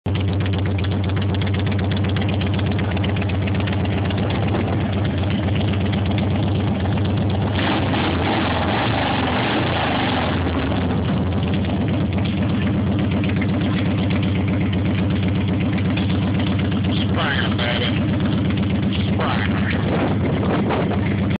HD - Springer softail - vance n' hines
looking better and sounding better